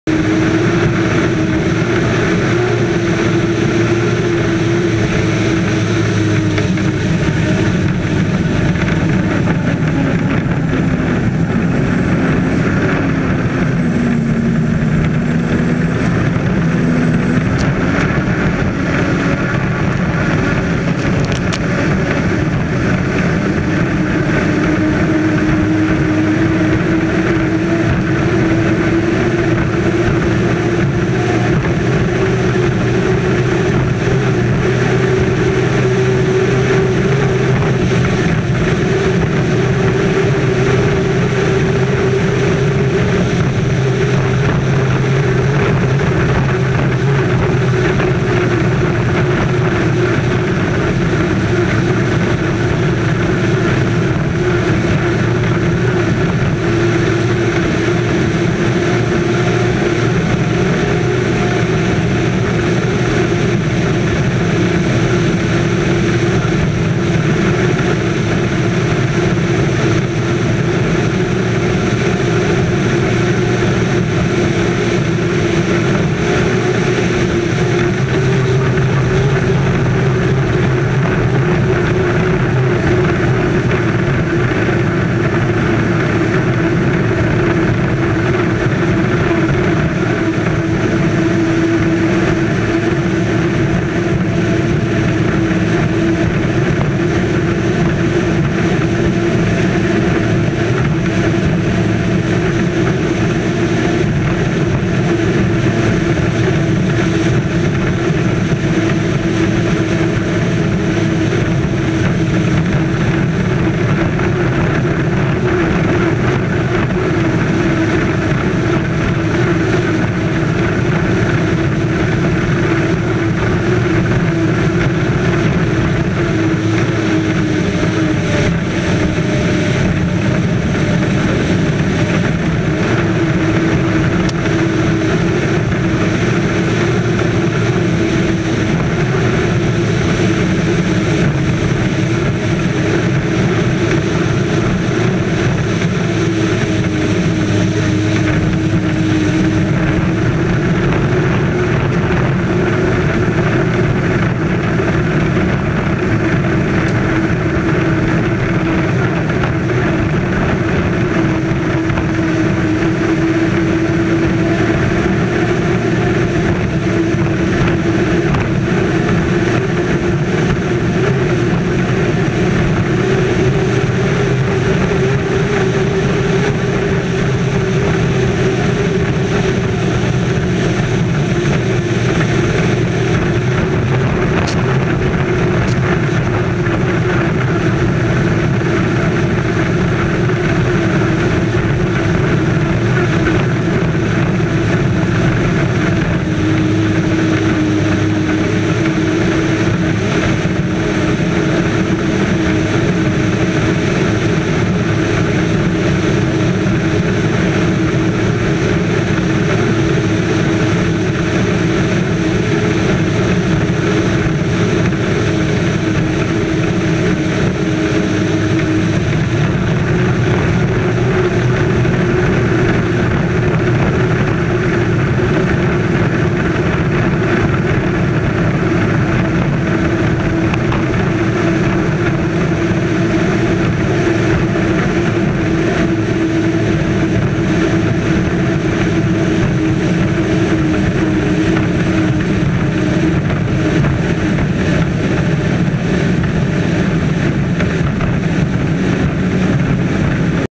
U-11 Peters & May: 2014 San Diego Bayfair Heat 2A
Onboard with Tom Thompson at the 2014 Bill Muncey Cup at Bayfair in San Diego, CA.